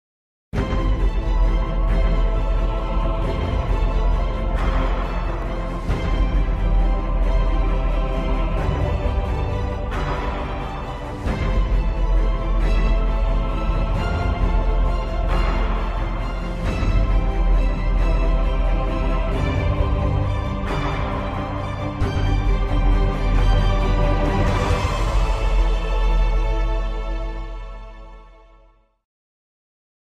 Intro Elemento Tango
Presentiamo in anteprima la clip video che accompagnerà i prossimi eventi di Elemento Tango - © 2019 Elemento Tango ASD - Visita e iscriviti alla Newsletter per ricevere in anteprima le informazioni sui prossimi eventi di Tango Argentino.